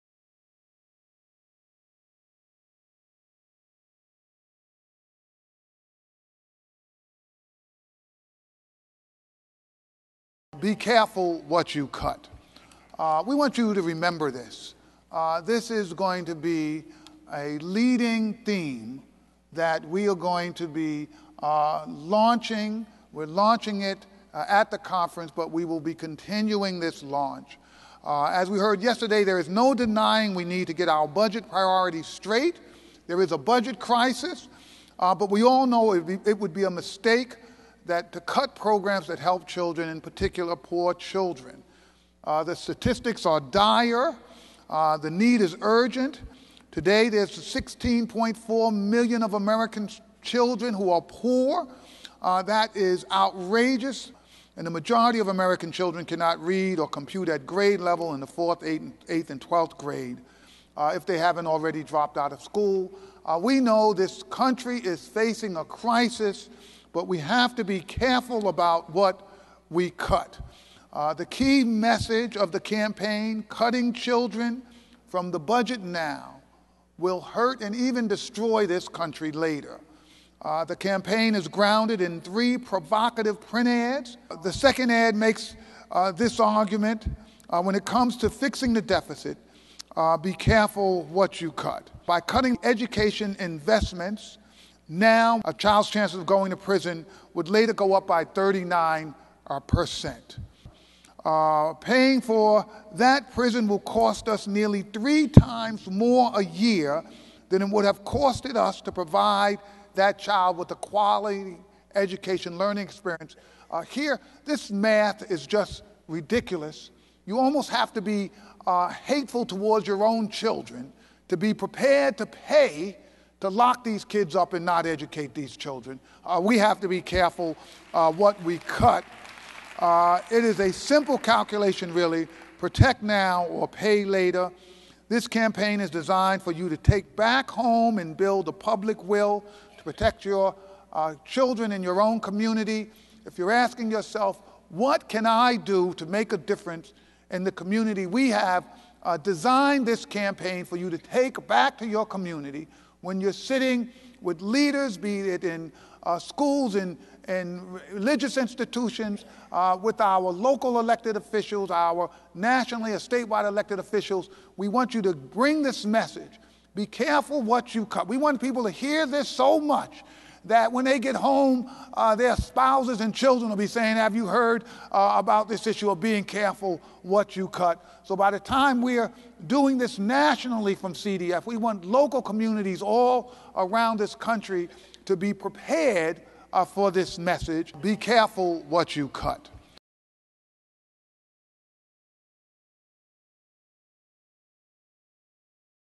Geoffrey Canada: Be Careful What You Cut
Geoffrey Canada, president and CEO of Harlem Children's Zone, Inc. and chair of CDF's board of directors, urged participants at the Children's Defense Fund National Conference to take action and join the Be Careful What You Cut Campaign, launched on Monday, July 23 in Cincinnati, Ohio. Canada talked about the dire statistics and the urgent need facing children, saying, "there is no denying we need to get our budget priorities straight." Cutting children from the budget now costs all of us later. Canada encouraged 3,000 plus advocates, organizers, researchers and young leaders to take the campaign back home and build public will in the community.